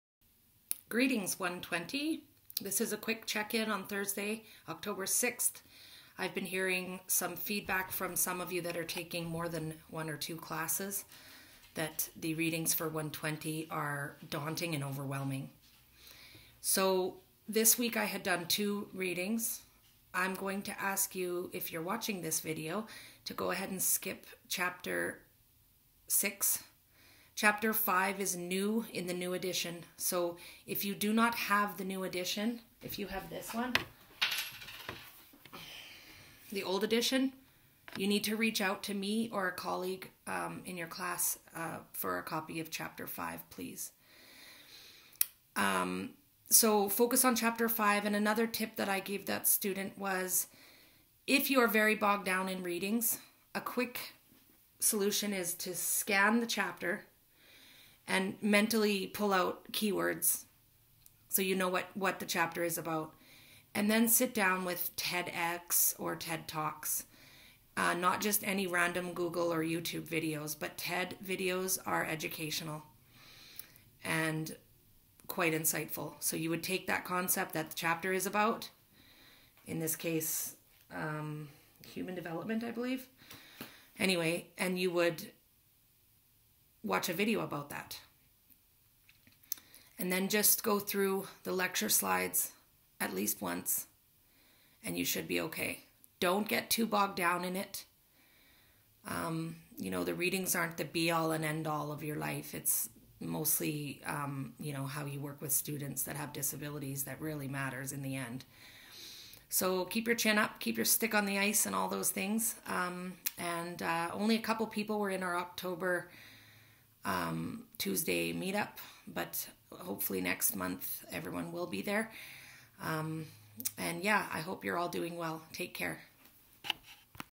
Val Lucas - October Readings
120 This is about your week 5 readings.
Are you a bit bogged down in the readings?